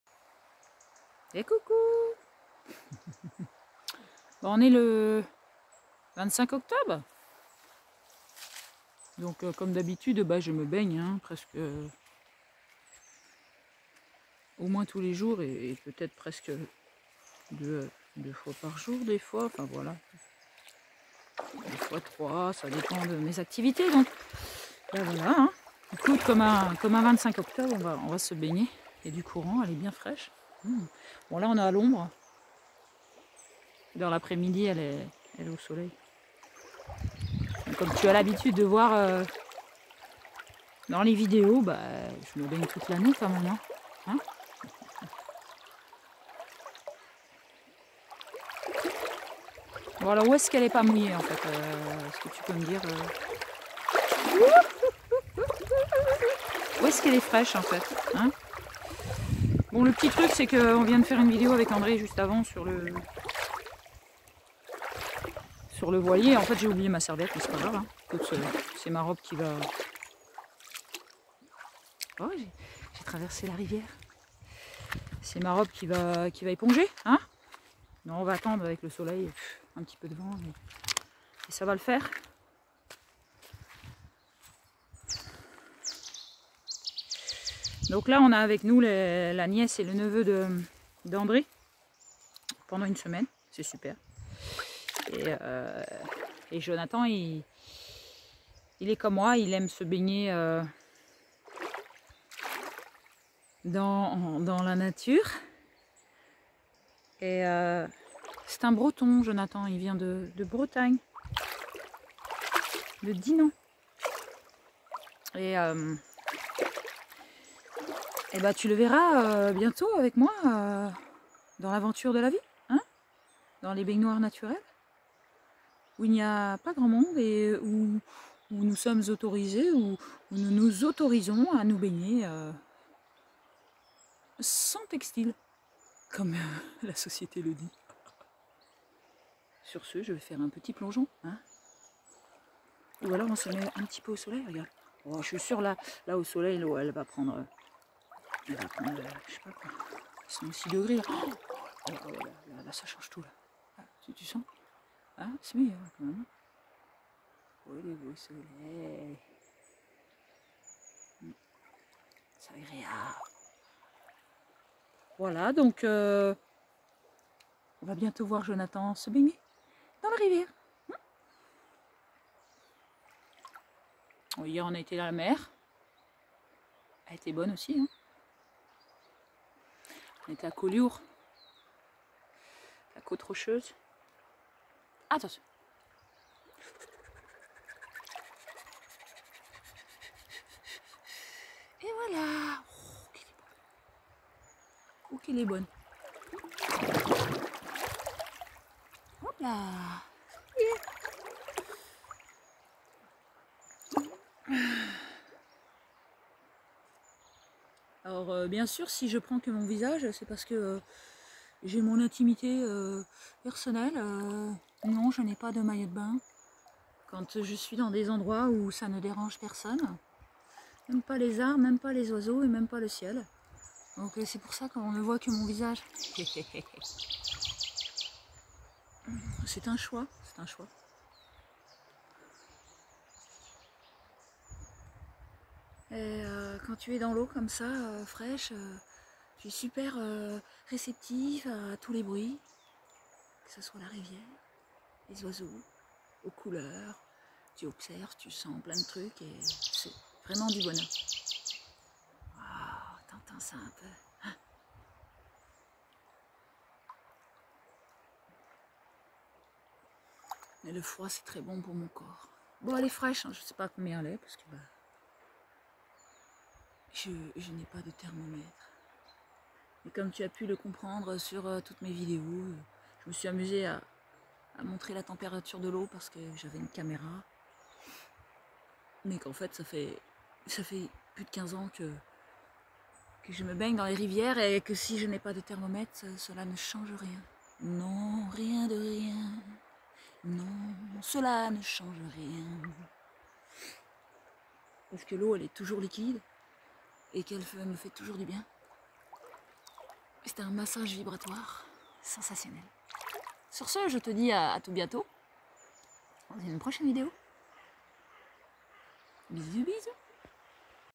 Baignade du 25 oct, et présentation de Jonathan bientôt sur les vidéos
sans textile ou baignade nue toute l'année c'est pour cela que je ne filme que mon visage !! lol !!
je réponds à la question. Merci à +
soutenir la chaîne :